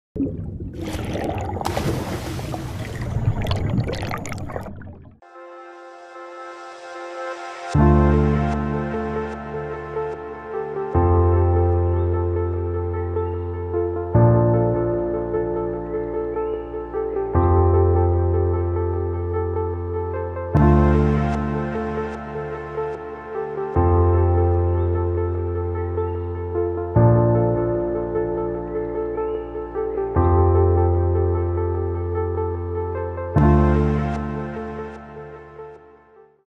Clown Trigger
Clown triggerfish (Balistoides conspicillum) never leave home without their makeup on. They are shy and solitary. While not as aggressive as their Titan cousin, they’re territorial and aren’t afraid to use their powerful jaws and teeth (which never stop growing), built for eating sea urchins, crabs, crustaceans and mollusks, which they uncover by blowing jets of water out of their mouths towards the sand.
They are one of the most colourful fish to encounter when diving and make for a brilliant photo op., but are unfortunately quite camera shy and generally scope you out (with eyes that can move independently) before swimming away.